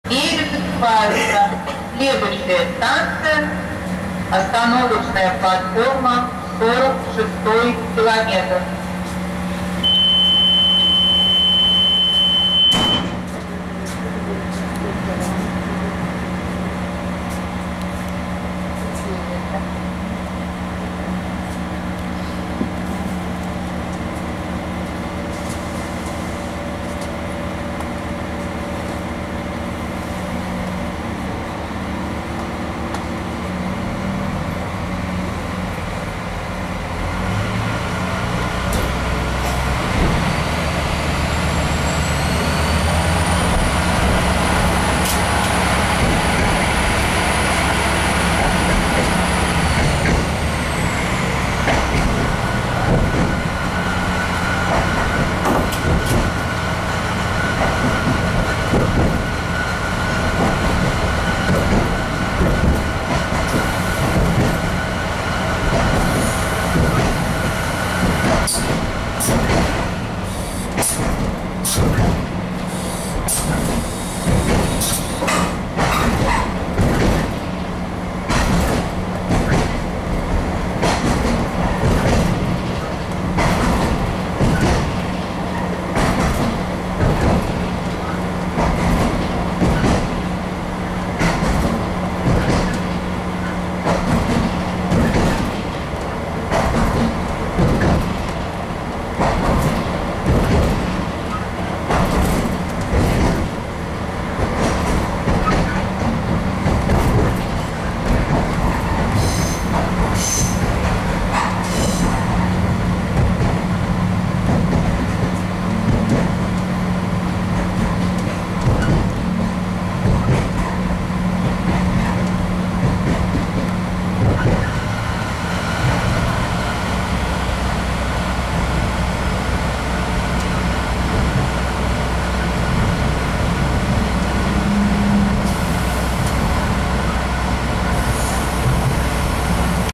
Отправление со станции Мукшур из окна рельсового автобуса
Отправление со станции Мукшур из окна рельсового автобуса сообщением Ижевск - Нижнекамск
Россия/Татарстан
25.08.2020